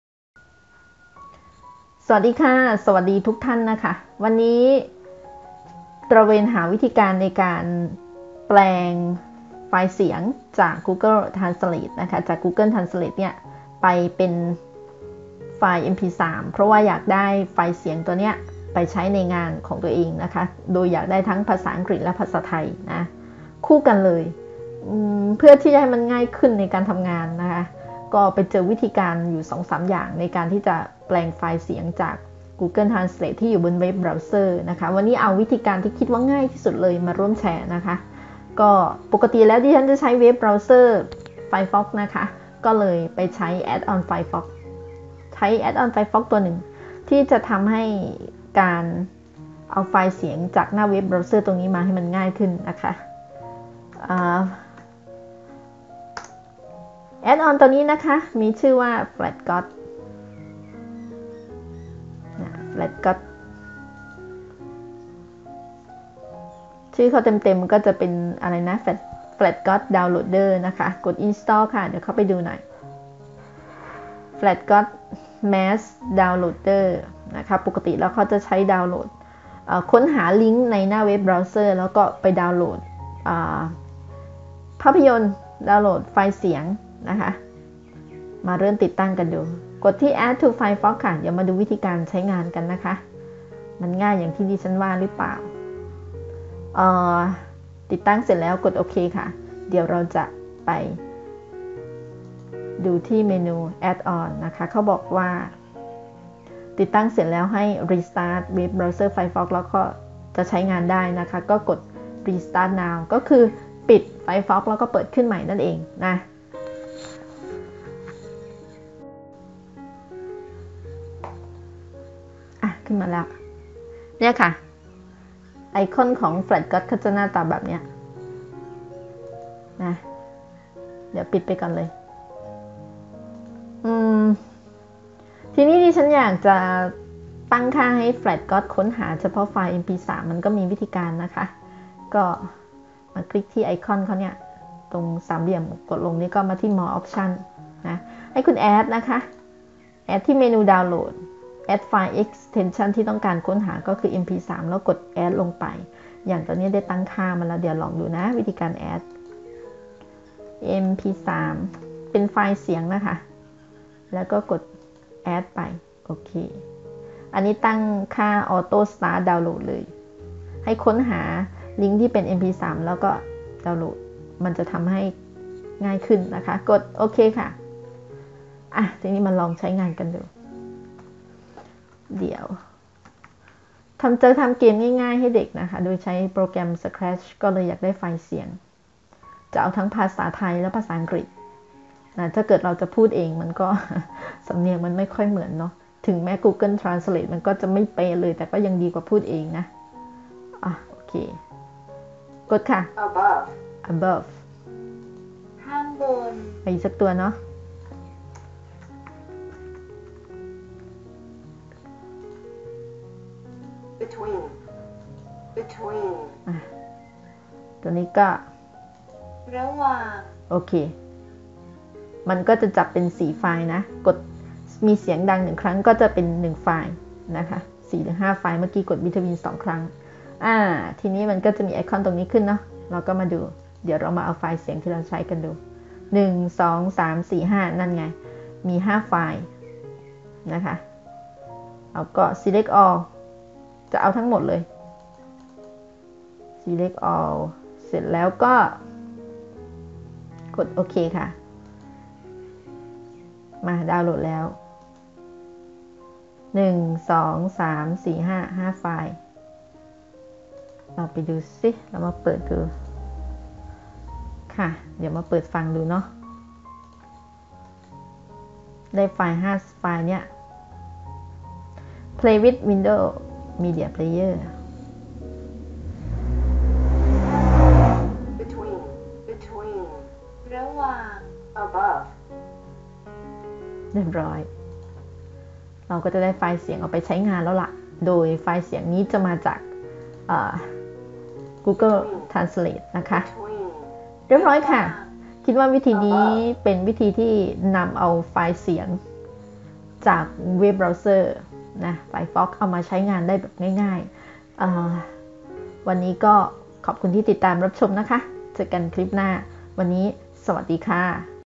แปลงไฟล์เสียงจาก Google Translate เป็น mp3
วิธีในการดาวนโหลดไฟล์เสียงจาก Google Transalte ในรูปแบบไฟลเสียง mp3 แบบง่ายๆ